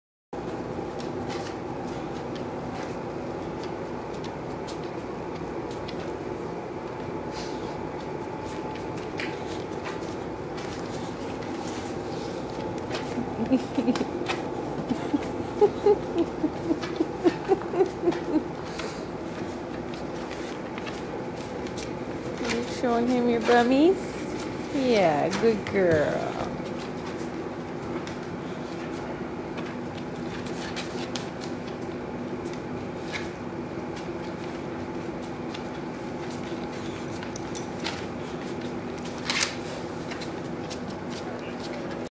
Ruby in her underwear
Boxer Ruby shows off her underwear to some of the dogs at her foster home. Ruby is in heat so the underwear is acting like a diaper. Ruby is in the care of HART All Breed Dog Rescue.